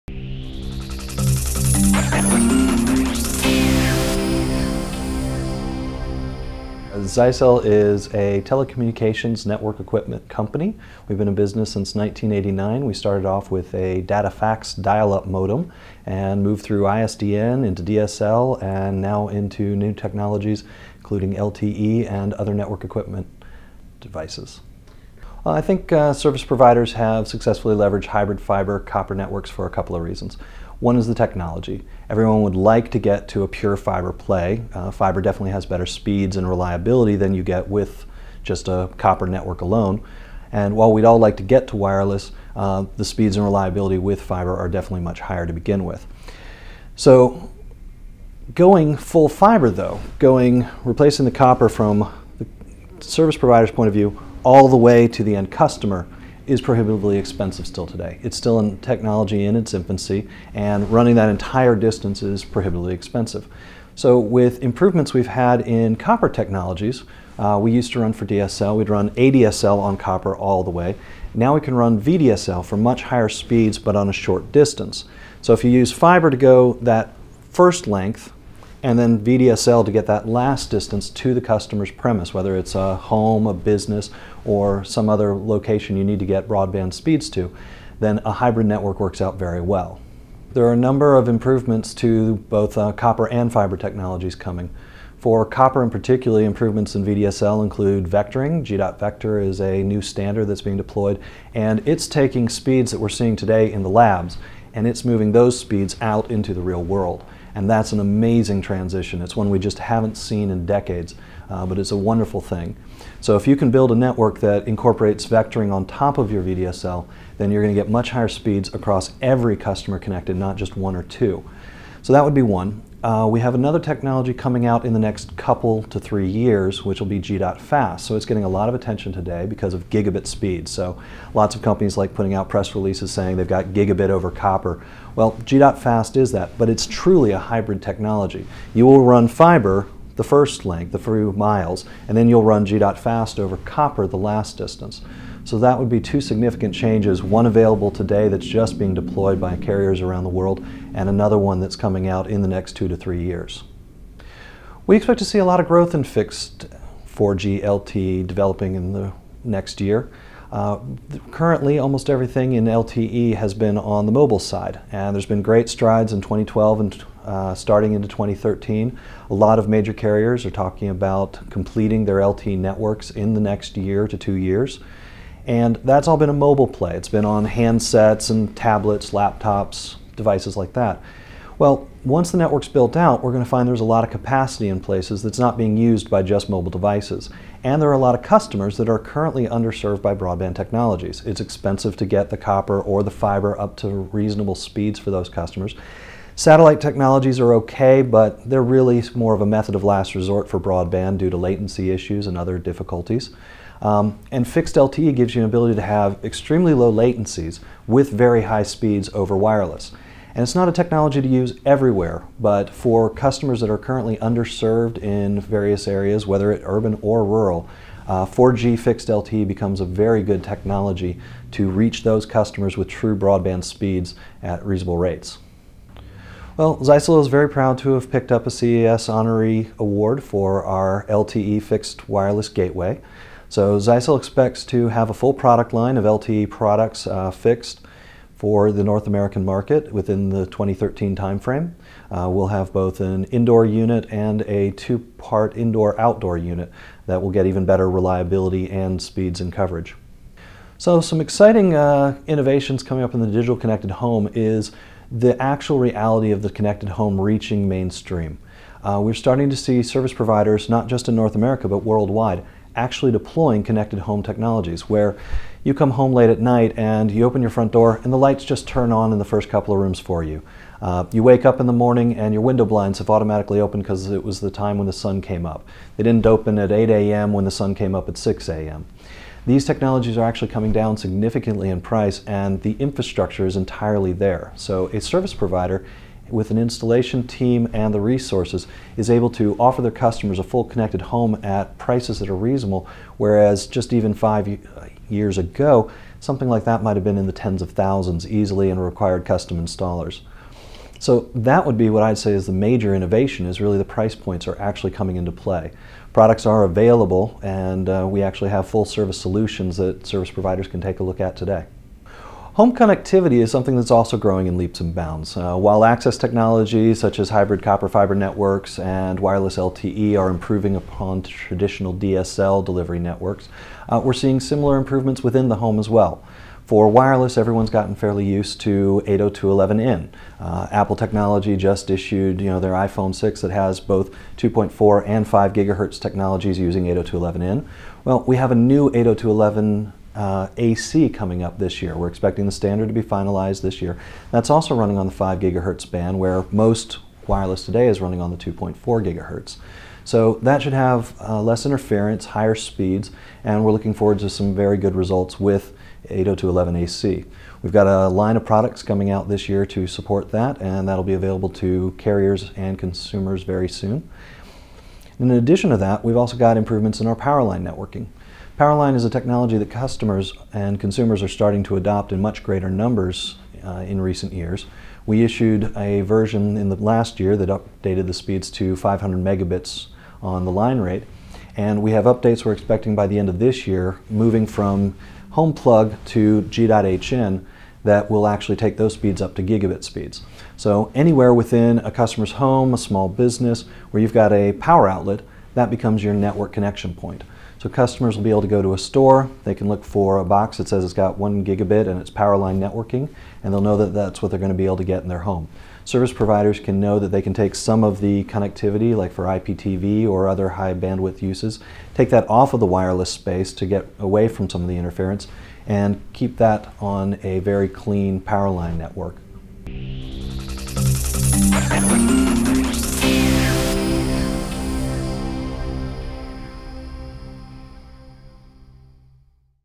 ZyXEL CES 2013
ZyXEL's Director of Product Management David Thompson, interviewed by TIA at CES 2013, discusses a variety of developments in technology affecting broadband and the communication industry in general. David also discusses new innovations by ZyXEL that will facilitate communications from service providers to their subscribers.